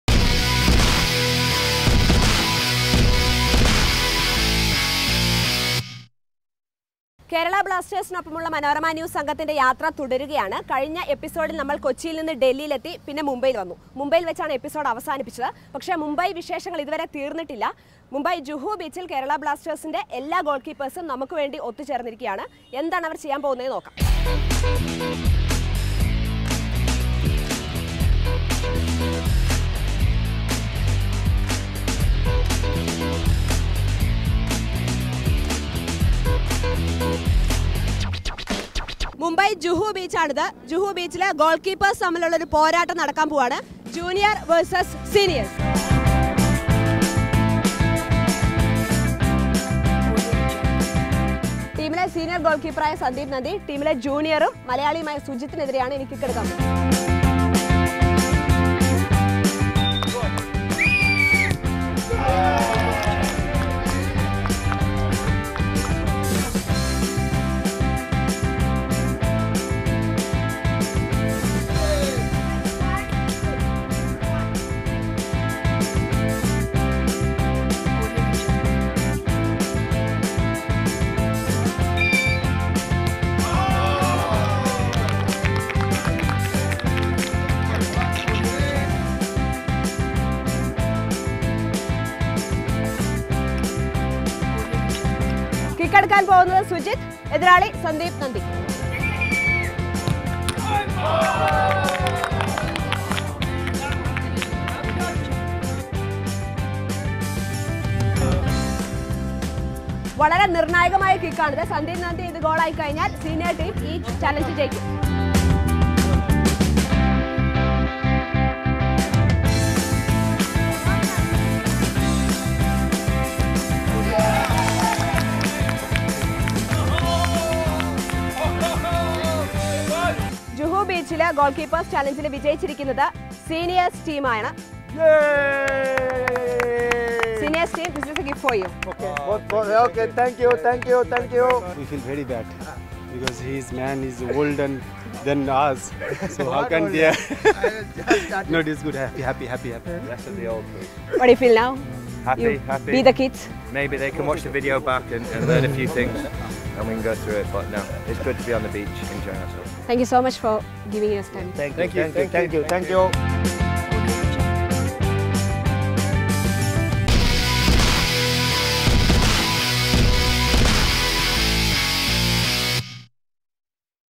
Junior GKs' vs Senior GKs' | Blasting Out with Kerala Blasters | Episode 4 Part 1
The Kerala Blasters junior goal keepers Sunjith Sasikumar and Subhasish Roy take on the senior keepers Paul Rachubka and Sandip Nandy on a penalty challenge at Juhu beach, Mumbai.

for the latest news about the club.

Kerala Blasters True Yellow Membership